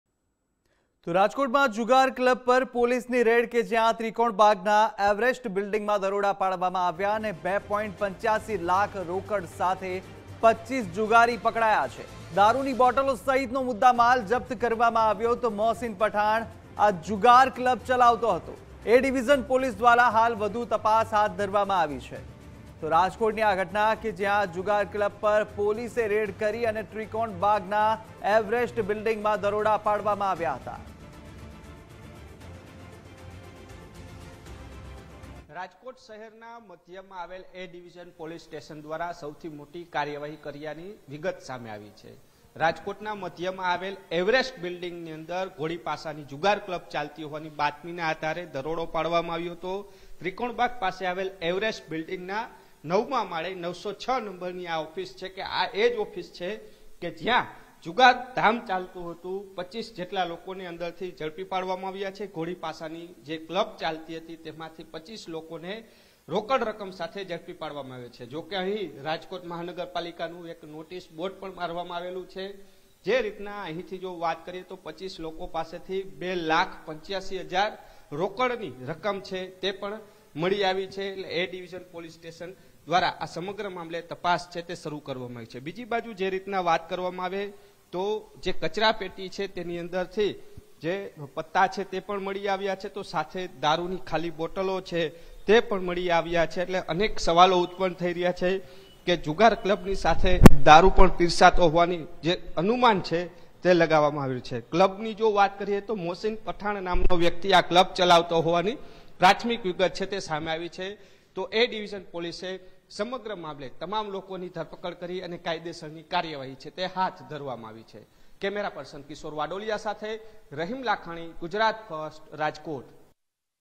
Rajkot: રાજકોટમાં જુગાર ક્લબ પર પોલીસની રેડ
રાજકોટમાં જુગાર ક્લબ પર પોલીસની રેડ
ત્રિકોણબાગના એવરેસ્ટ બિલ્ડીંગમાં દરોડા
2.85 લાખ રોકડ સાથે 25 જુગારી પકડાયા
દારૂની બોટલો સહિતનો મુદ્દામાલ જપ્ત કર્યો
મોહસીન પઠાણ ચલાવતો હતો જુગાર ક્લબ
એ ડિવિઝન પોલીસ દ્વારા વધુ તપાસ ચાલુ